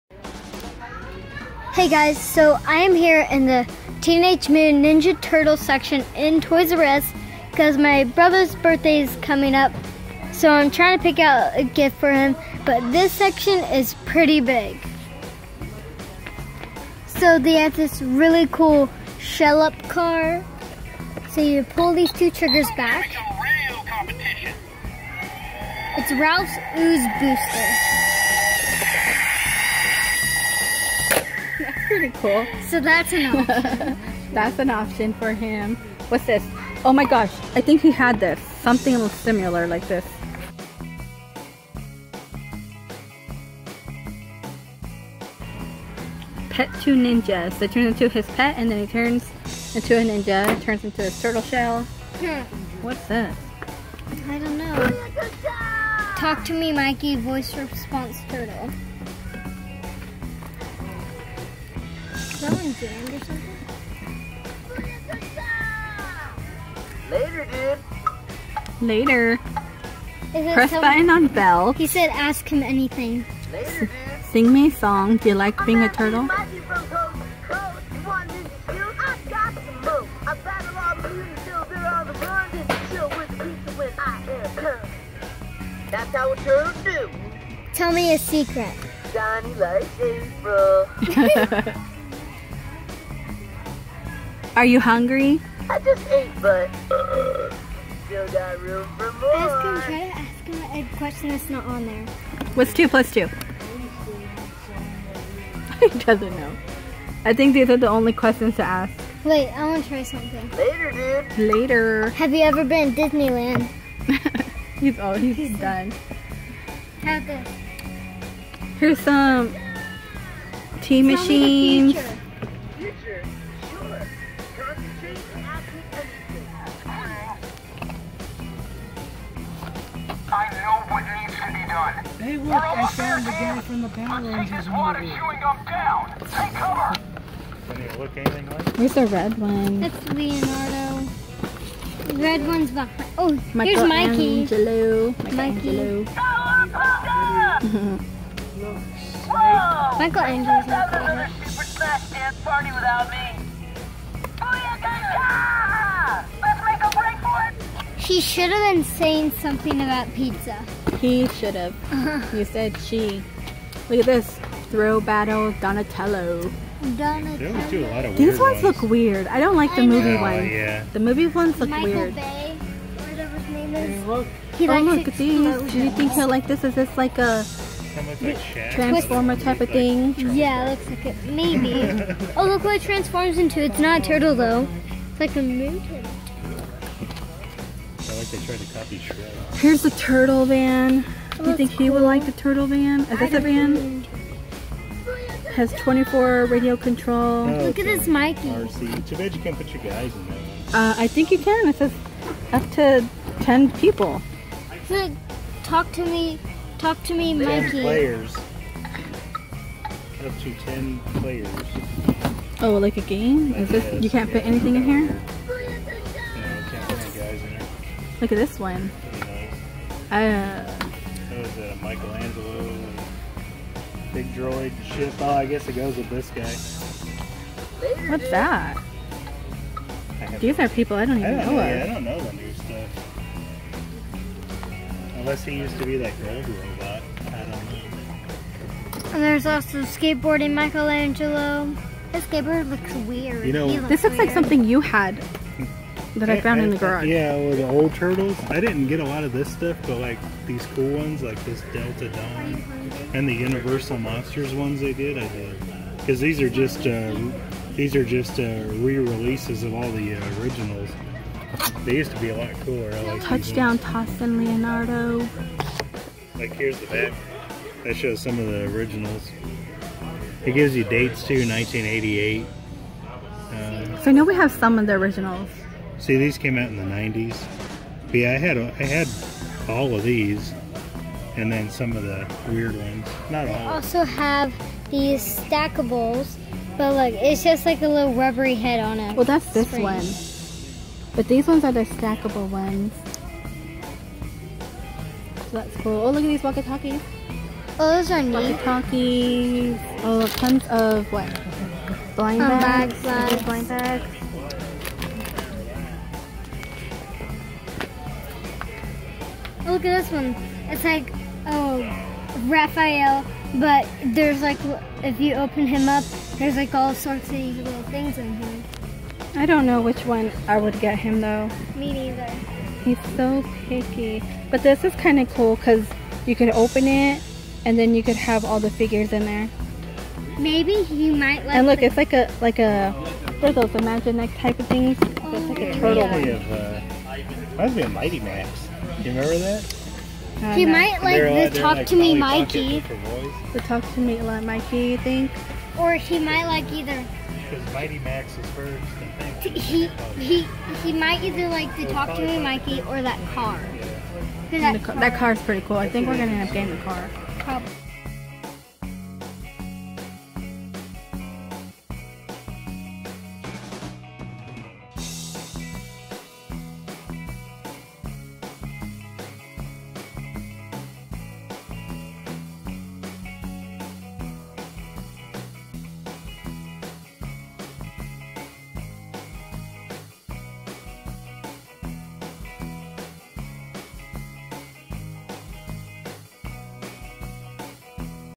Teenage Mutant Ninja Turtles Toy Hunt Toys R Us Raphs Ooze Bosster talking mikey 2017
Hola Guys!

My little brother is turning 5 he is in love with Teenage Mutant Ninja Turtles. I will say I don't know much about them either does my mom. So I think we say things wrong lol but it's ok because toy hunts are fun and not meant to be scripted. Anywho they have tons at toys r us . They have t machines of ninja turtles and action figures , Raph, mikey,leo,etc,, Shop with us for a birthday present :)

Toy hunt at toys r us

 I like to have fun and share.
Toy hunting with my family is fun. I also love to share my dolls and lip balm.

BYE!!!

yay summer is here !